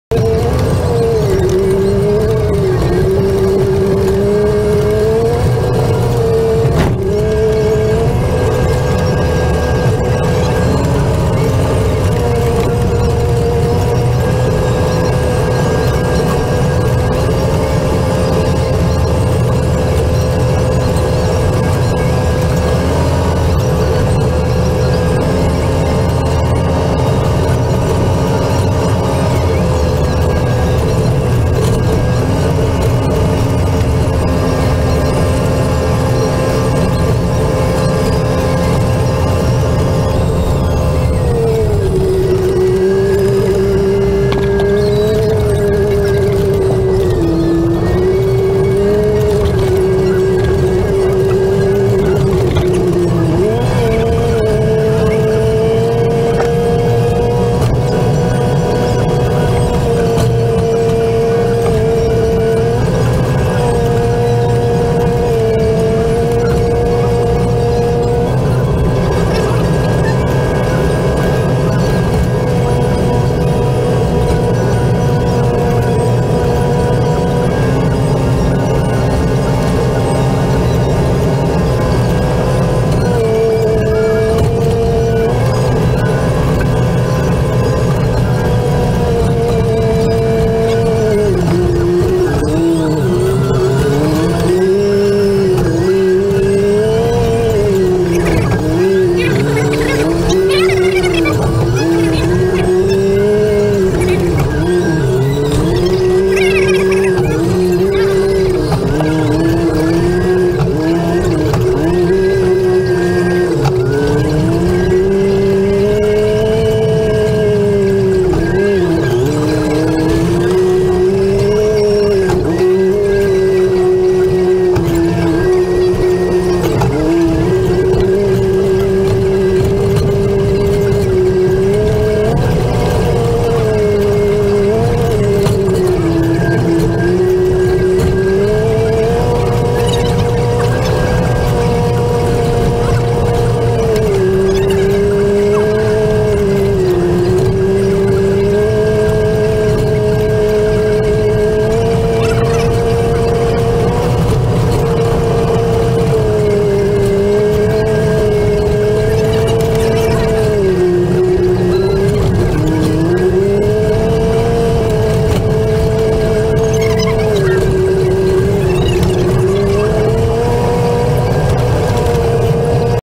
Metsätien aurausta
Traktorina John deere 6420 ja vama j130 perälevy.